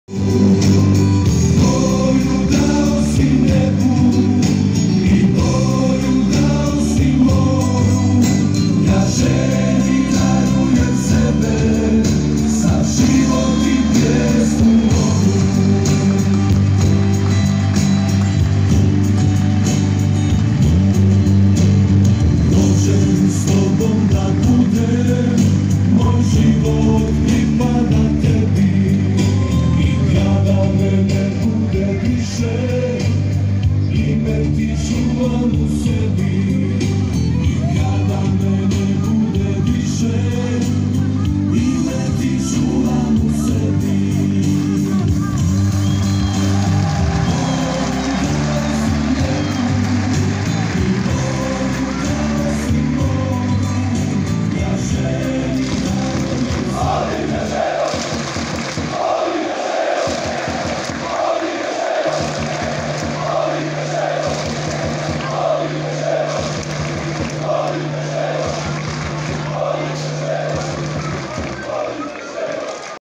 Navijači Željezničara pozdravili igrače nakon remija protiv Borca
Željezničar je u prvom kolu Premijer lige Bosne i Hercegovine remizirao protiv Borca na "Grbavici" rezultatom 1:1.